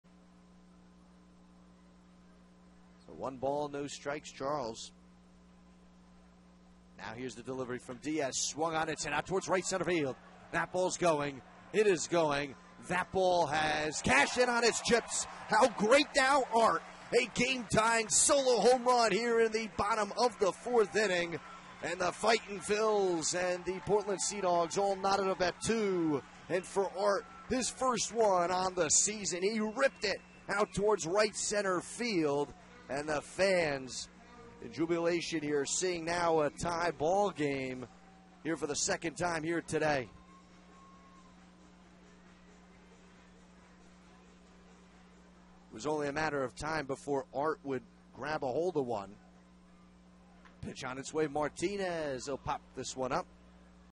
Reading's Charles goes yard
5/03/15: Reading's Art Charles belts a solo shot as the Fightins top Portland, 6-5.